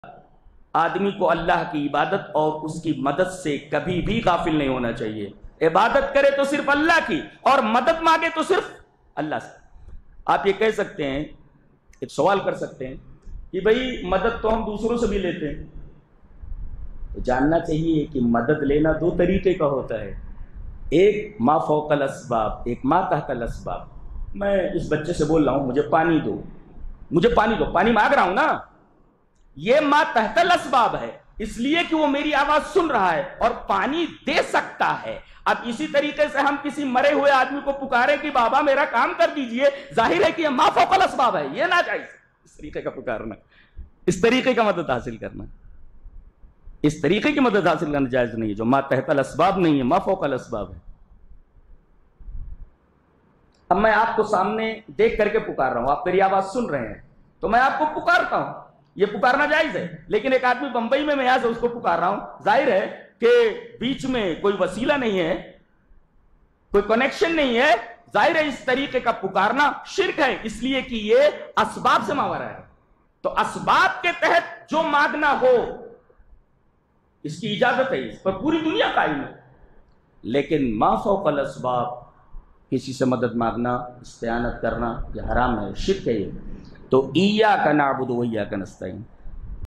Madad Mangne ka 2 Tareeqah | kaunsa Jayez? | Allama Jalaluddin Qasmi
مدد مانگنے کی دو شکلیں...لیکن جائز کونسی والی؟
ویڈیو دیکھیں.